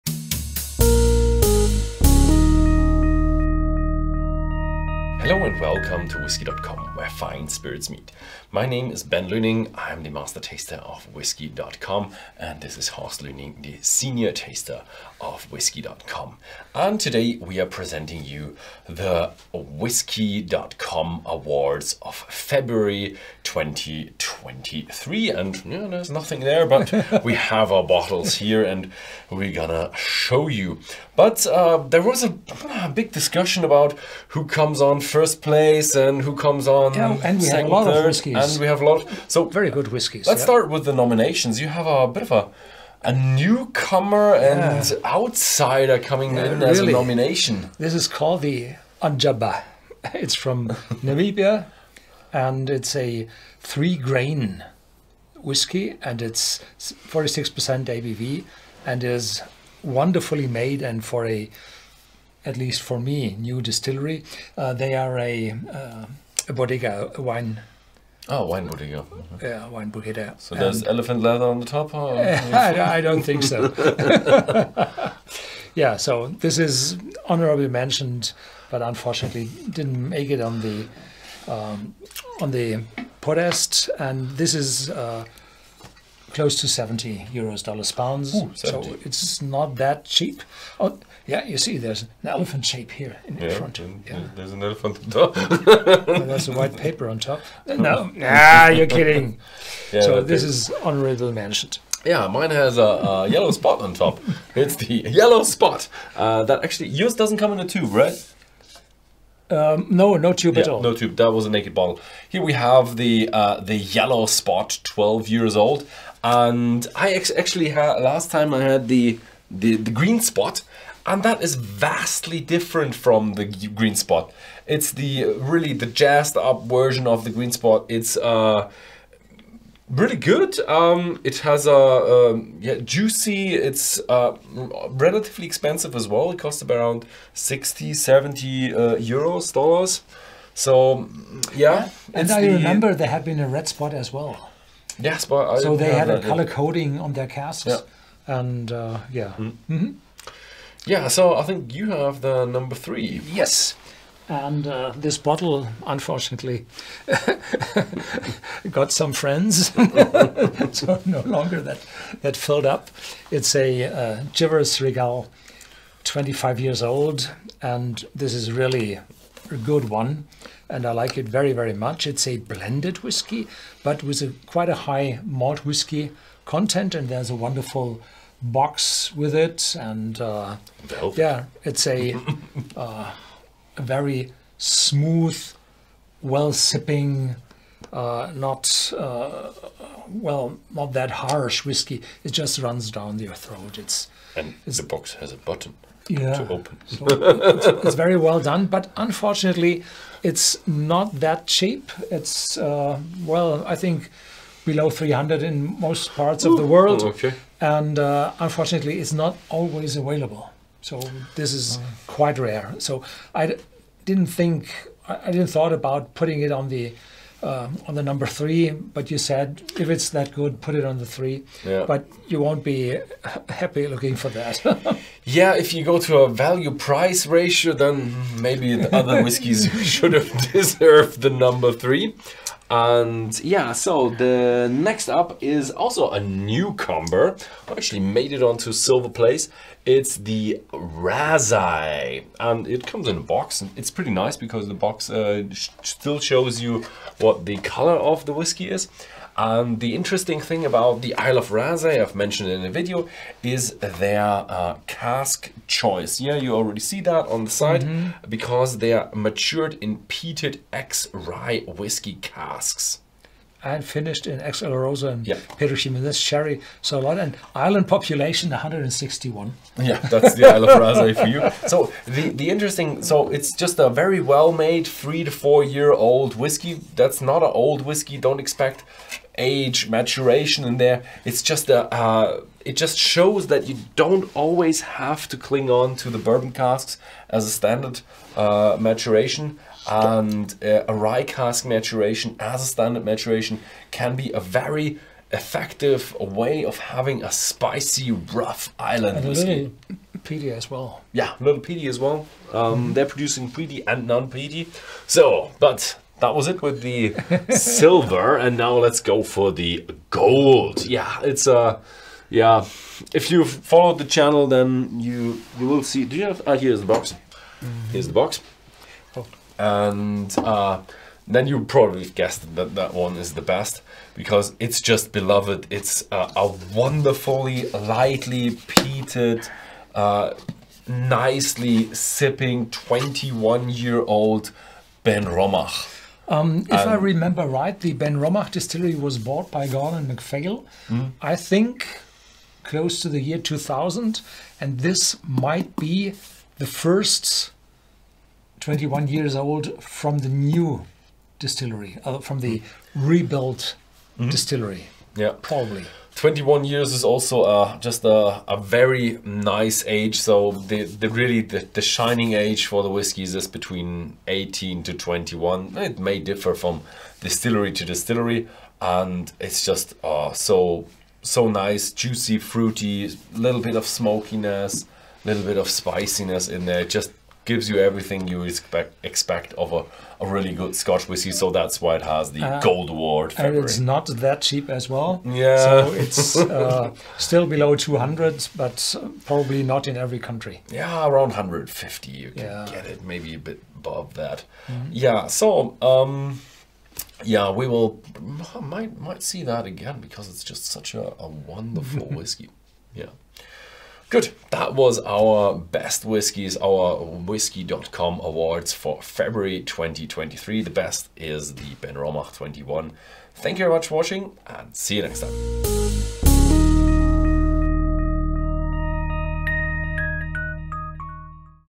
Whisky.com Award February 2023
Ben and Horst Luening present the best whiskies of february 2023. The selection is made from all whiskys that Ben and Horst Luening tasted in february.

Honorable mentions:
01:16 Ondjaba Triple Grain
00:42 Yellow Spot 12 Years

Winner:
02:06 Chivas Regal 25 Years
03:01 Raasay Single Malt - Sherry Finish 1st Release
03:53 Benromach 21 Years